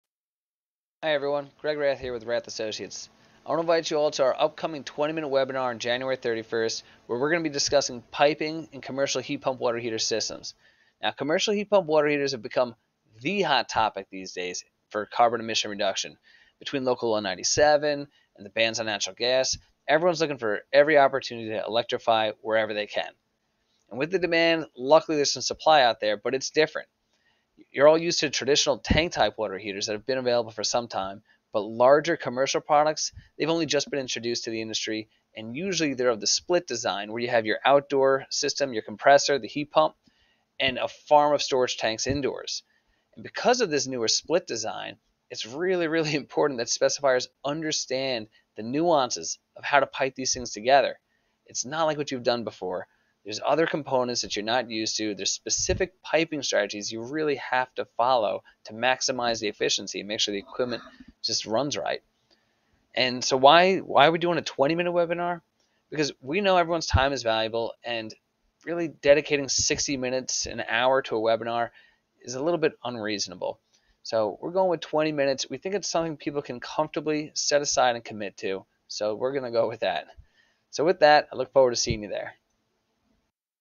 Rathe Webinar Series Hot Water Heat Pumps Piping
Join us for our first part of the Rathe Webinar Series on Hot Water Heat Pumps.  Gregory Rathe will discuss the importance of piping for the Hot Water Heat Pumps to make it cost effective and efficient.